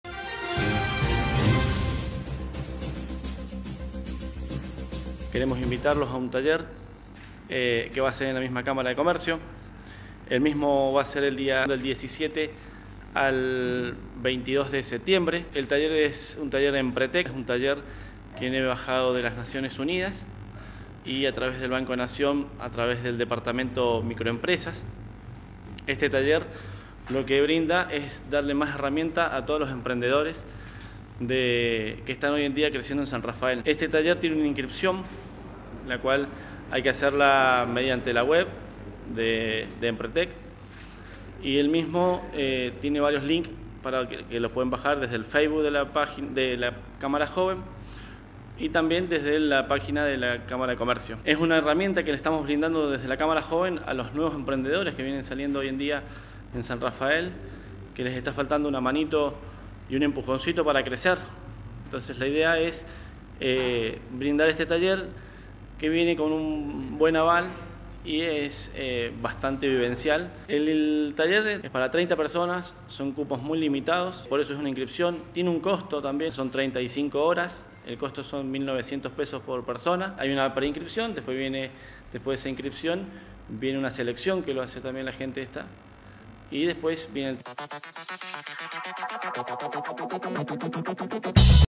TALLER EMPRETEC
Ser realizará desde el 17 al 22 de septiembre en la Cámara de Comercio de San Rafael. Una buena oportunidad para emprendedores.